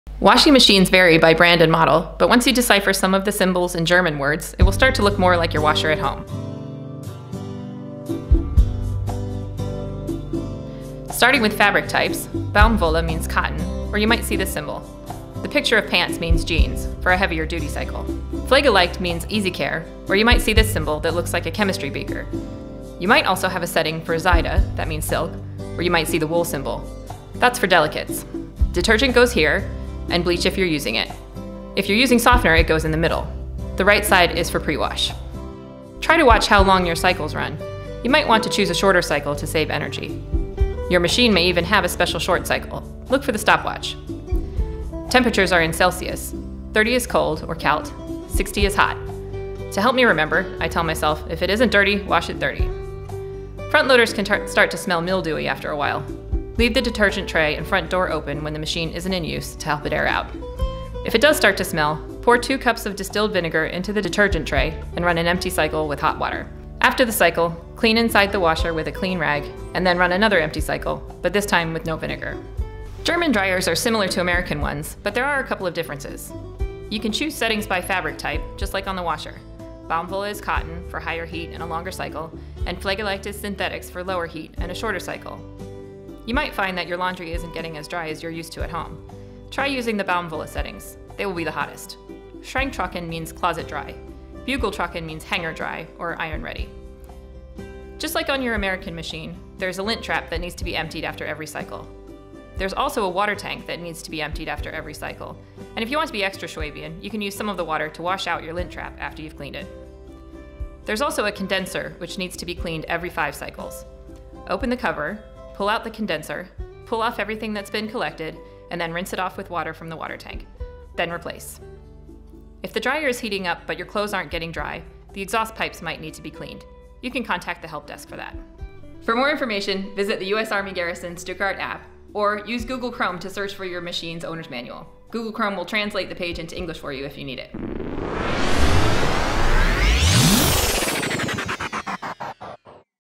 Using Your Washer and Dryer in Germany
Laura Love DPW Business Operations Specialist takes us through some common settings and features for European Washers and Dryers.

Produced and/or uploaded by the U.S. Army Garrison Stuttgart Public Affairs Office.